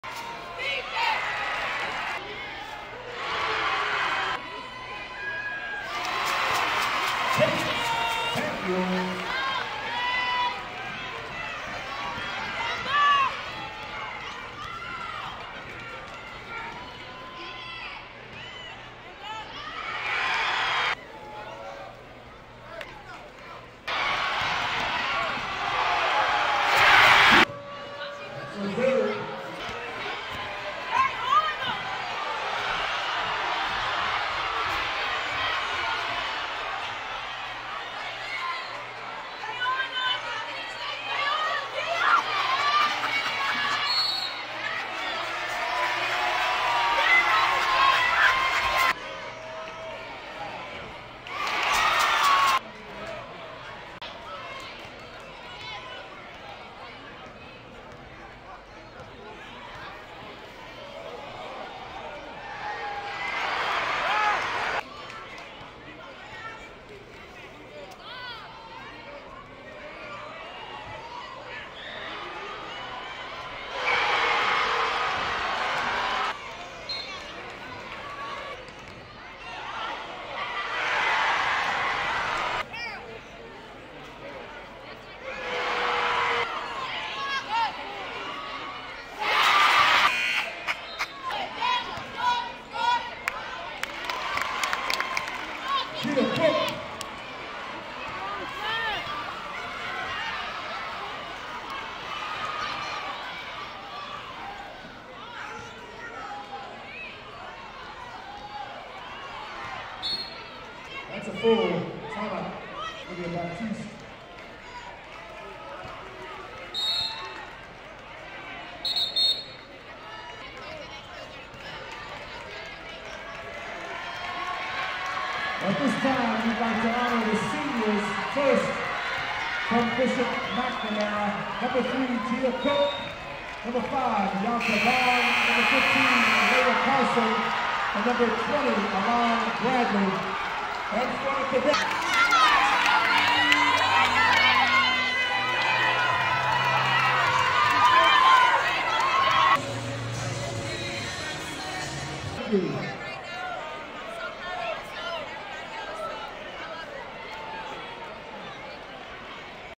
2022 WCAC Girls Championship Kyndal Walker & Isabella Tanedo Vs Gia Cooke (St Johns Vs McNamara)
Kyndal walker and Isabella Tanedo pulled Off a WCAC Championship Win  Against Gia Cooke's Mustangs